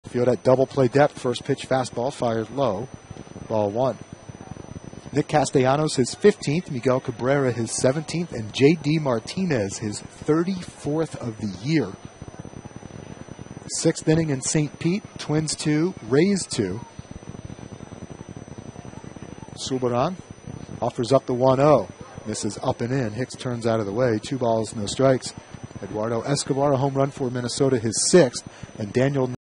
Ryan Rua homers for the Express
8/26/15: Ryan Rua crushes a three-run homer in Round Rock's 7-5 loss to Salt Lake